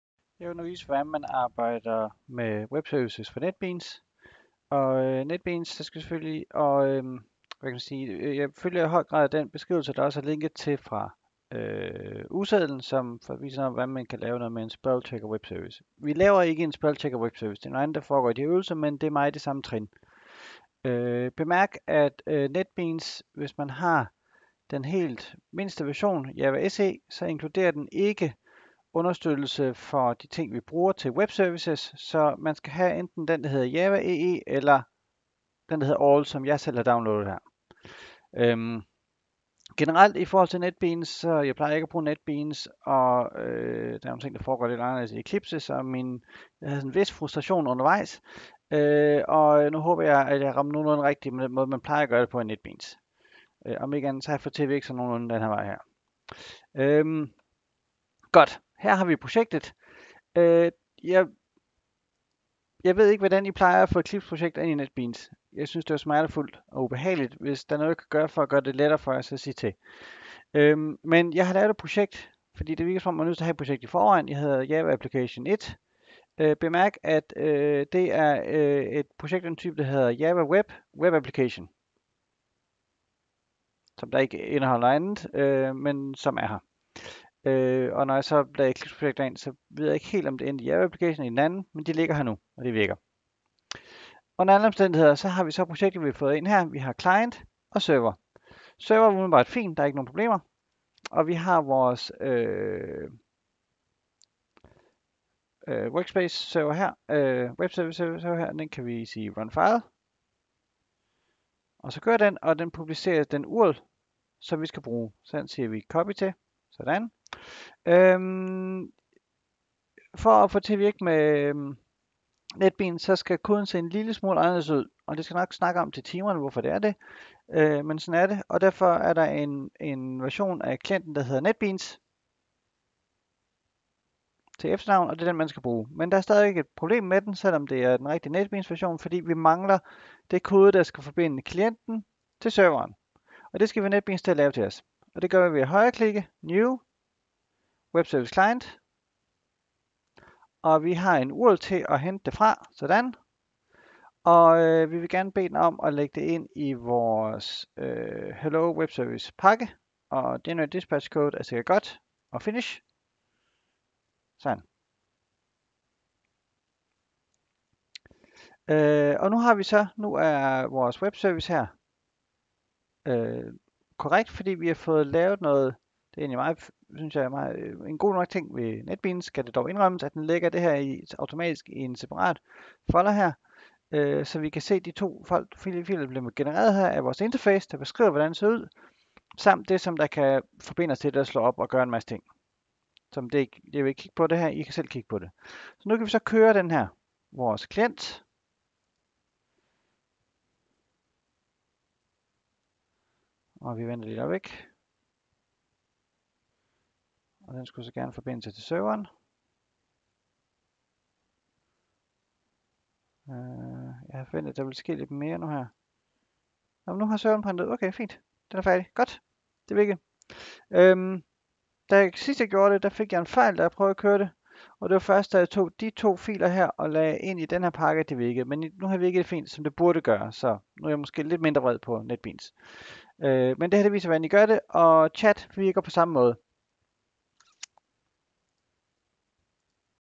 RPC: working with web services in netbeans
Quick walkthrough of how to use web services with netbeans, using the "hello world" example.